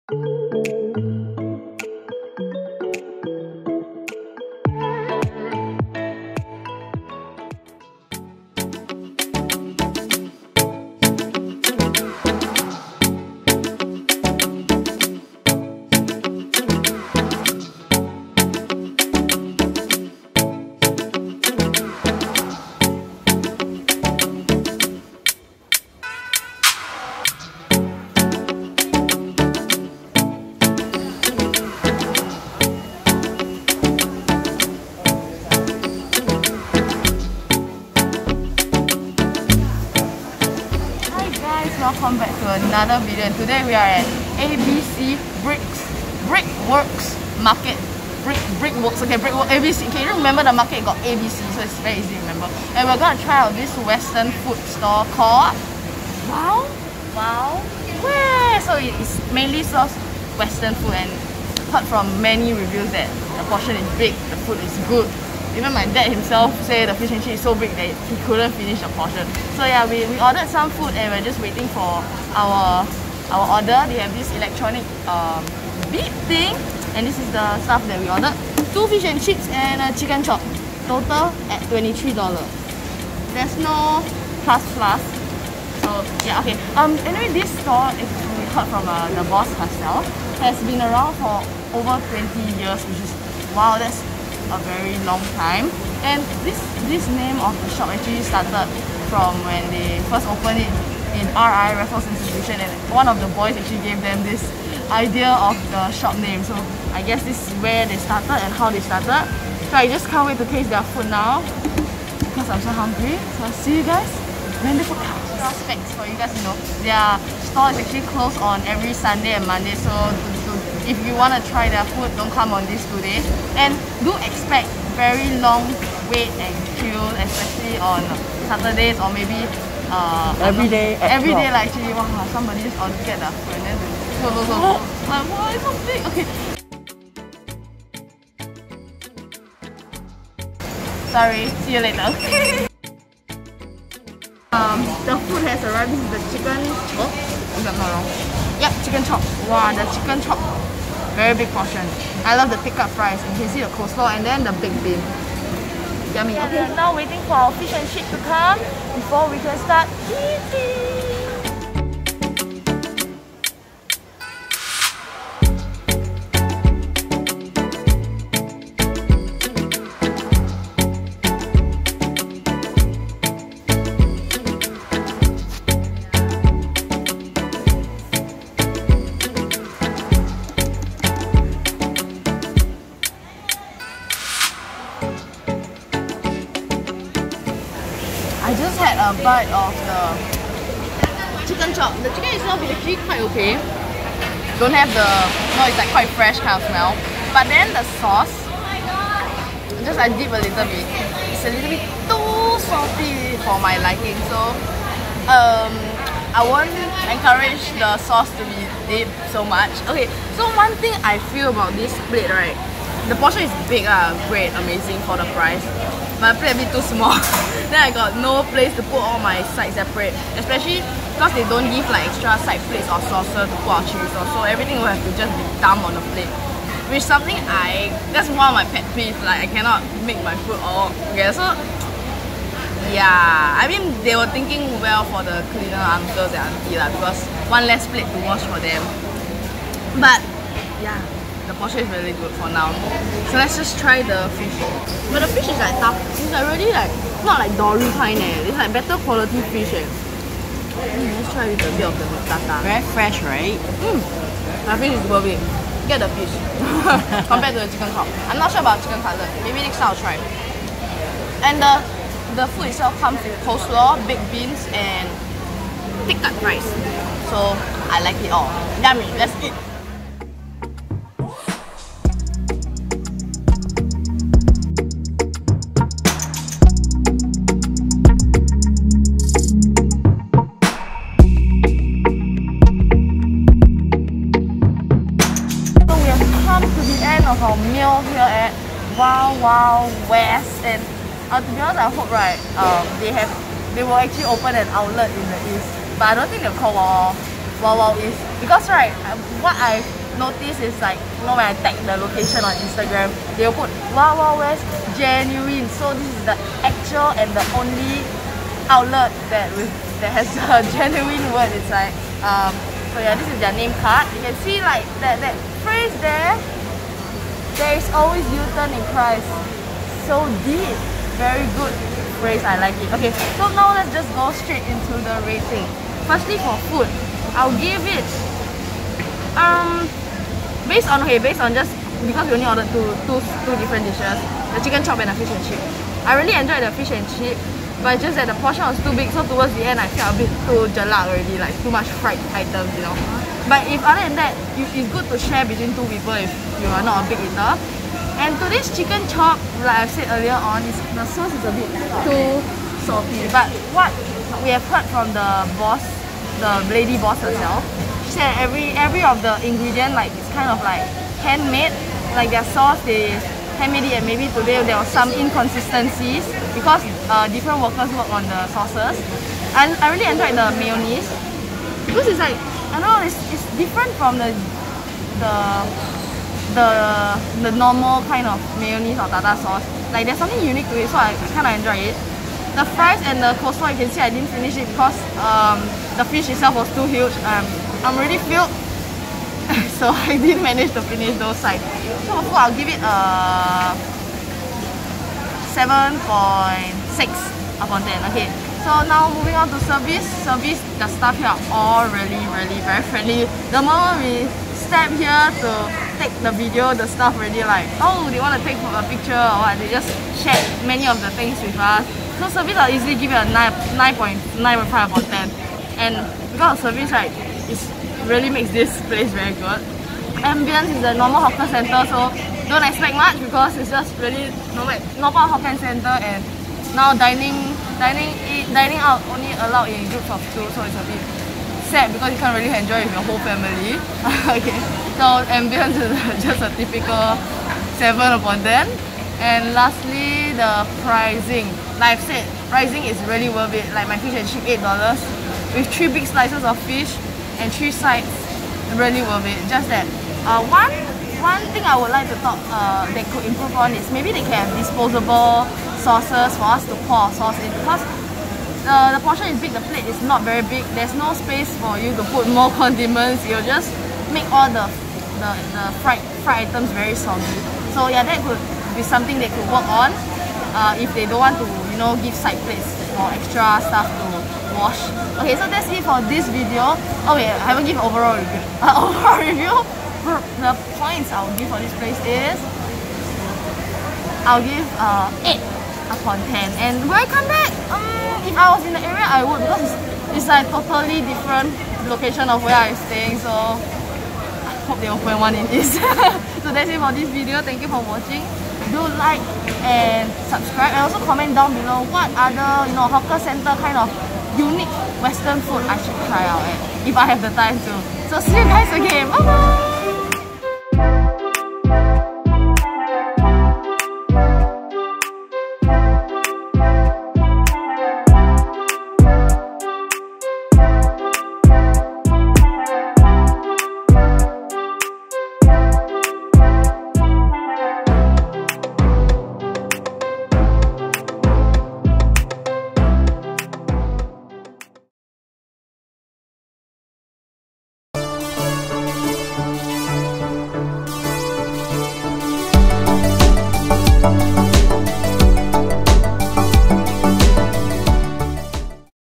WOW ... Wow Wow West GENUINE Large in Size, Value for Money at ABC
WOW WOW WEST GENUINE

ADDRESS:
Stall no 01-133 - ABC Brickworks Food Centre, 6 Jalan Bukit Merah, 150006

NEAREST MRT: Queenstown

Wow Wow West Genuine serves a variety of homemade, delicious Western Food Under S$10.00.  It has a much simpler menu,  serving a range of eight items Only.

They supported the Yellow Ribbon Project by hiring ex-offenders and ex-drug addicts to work at its branches.

Their first stall was at Raffles Institution, which was named “Wow Wow West” by the boys, before moving to ABC Brickworks.

Our very own Recommended item...
their Fish & Chips 鱼扒套餐 (S$8)👍👍👍👍👍

To get to know more about them, click on the link below;

🌐 More information can be found on their website and/or Facebook & IG;

[  ] IG account: Wow Wow West Genuine

Opening Hours:
TUESDAY - SATURDAY
10:30am - 2:00pm
4:30pm – 8:30pm
Closed: Every Sundays & Mondays
* Best to avoid Lunch & Dinner time!

Activate the bell 🔔 to receive all notifications.

☑ All review is based on our own experience

🎶All copyright goes to the owner of the music played in the background, no infringement is intended.